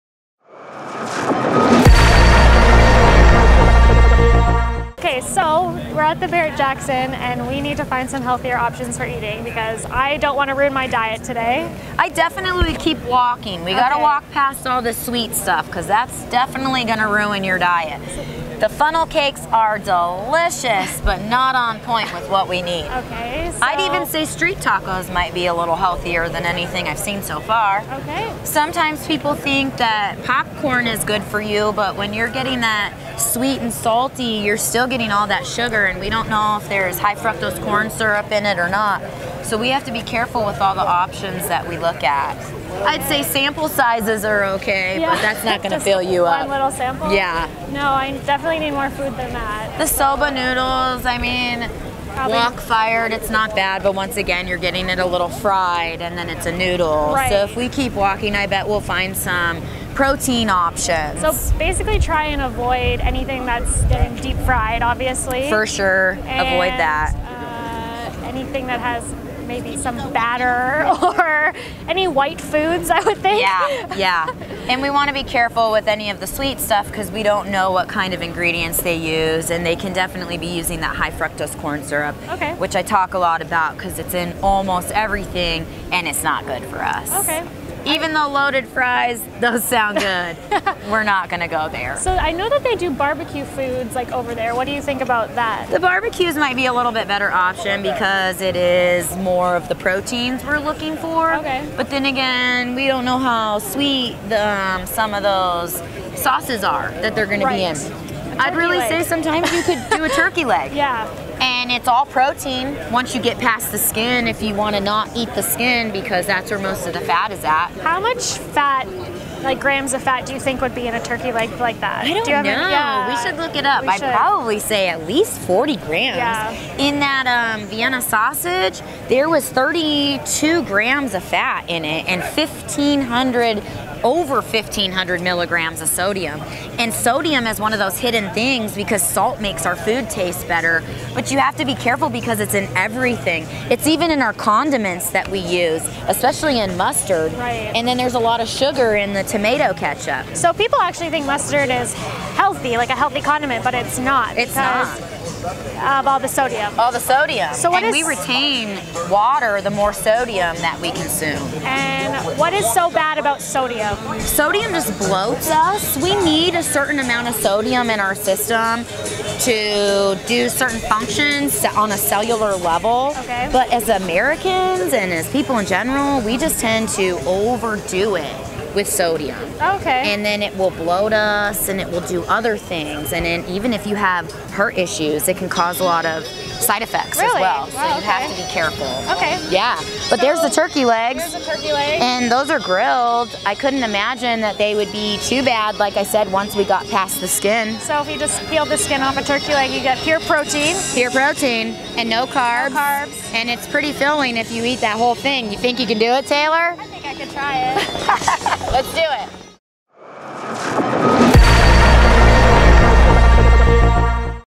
Healthy Eating at Barrett Jackson Car Show 2020 Part 1
Healthymale attended the Barrett Jackson Car Show in Scottsdale Arizona. In this video we walked around and checked out all the food options and narrowed it down to what we feel is the healthiest food option for you at the Barrett Jackson Car Show 2020 in Scottsdale, Arizona.